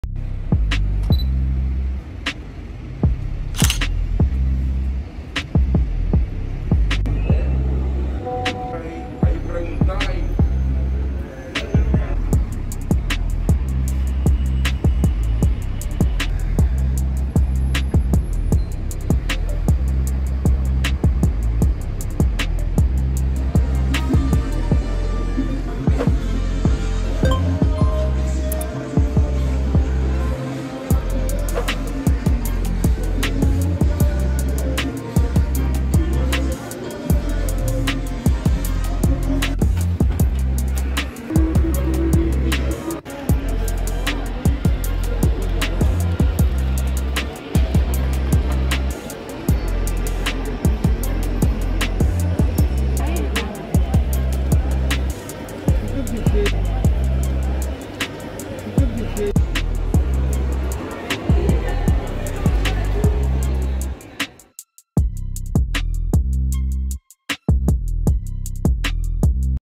En 1 minuto: visitamos el shopping y terminal Tres cruces - Montevideo
Hola, como estan todos!, soy alcon.ph y en este video recorrimos el shopping y terminal de omnibus Tres Cruces de Montevideo